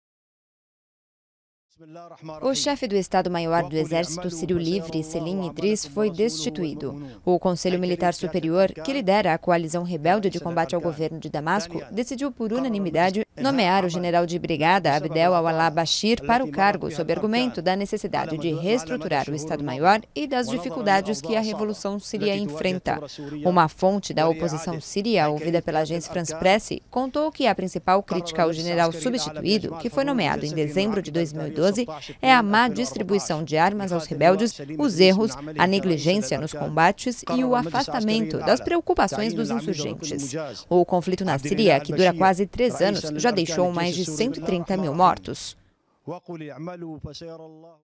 Exército Sírio Livre destitui comandante
O Conselho Militar Superior, que comanda a coalizão contra o regime da Síria, destituiu o chefe do Estado-Maior do Exército Sírio Livre. Fontes da oposição disseram que alguns dos motivos foram a má distribuição de armas aos rebeldes e a negligência nos combates.